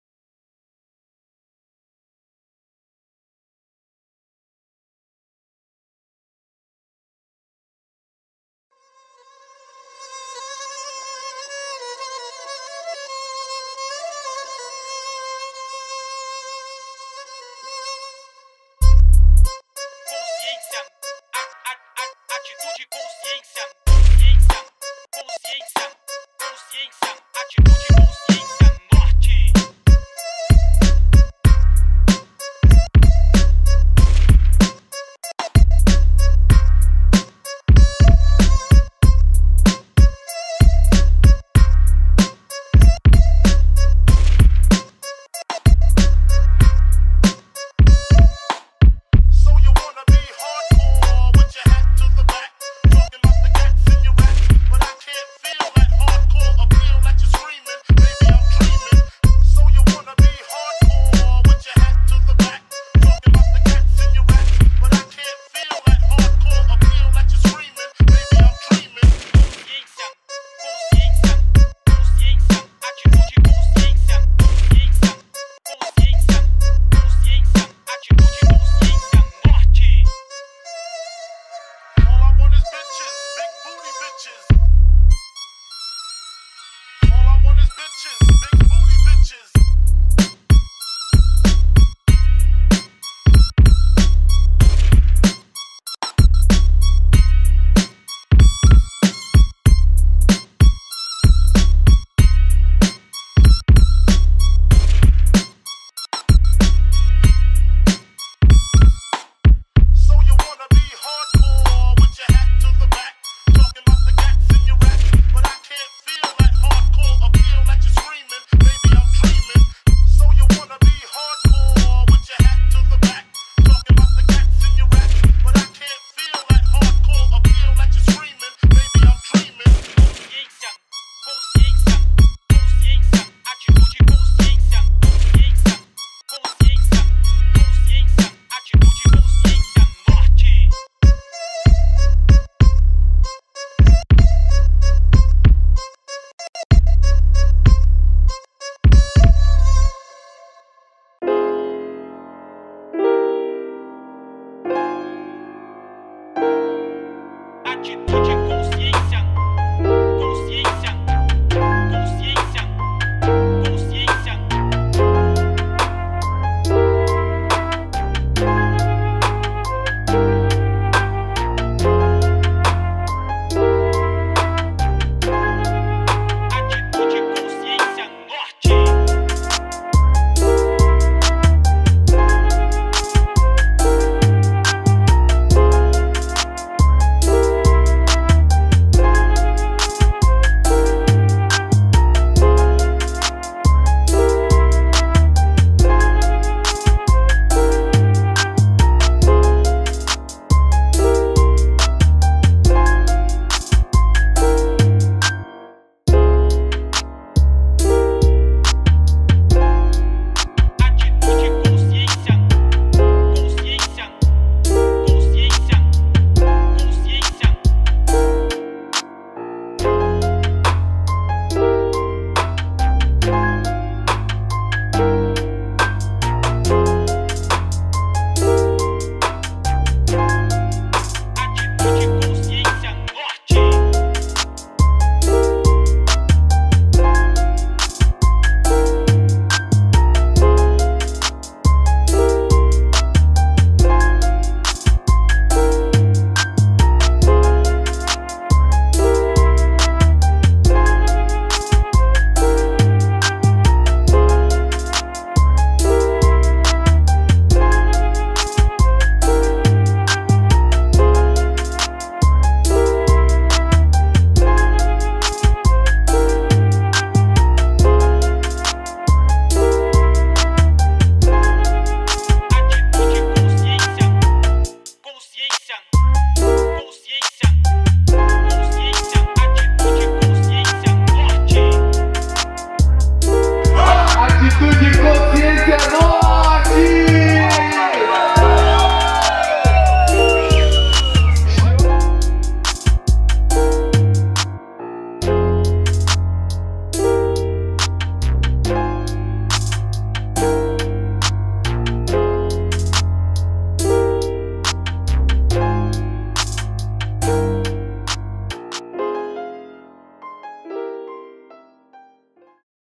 ATITUDE E CONSCIÊNCIA NORTE ED. VILA BIQUINHA-PLANALTO 2019
A 15 ANOS MOVIMENTANDO O HIP HOP NA ZN, EM 2019 NÃO FOI DIFERENTE, EM MAIS UMA EDIÇÃO NA VILA BIQUINHA-PLANALTO, O ACN FOI RECEBIDO PELA COMUNIDADE COM MUITA ALEGRIA.PRODUÇÃO MUSICAL: UAI SOM PRODUÇÕES, POR: FL1NT
PRODUÇÃO AUDIOVISUAL: (P.DRÃO VÍDEO CLIPES E VIRA FILMES )

REALIZADO COM RECURSOS DA LEI MUNICIPAL DE INCENTIVO À CULTURA DE BELO HORIZONTE. FUNDAÇÃO MUNICIPAL DE CULTURA E A MGS.

VISITE: FACEBOOK.COM/ACNBH (ATITUDE E CONSCIÊNCIA NORTE!).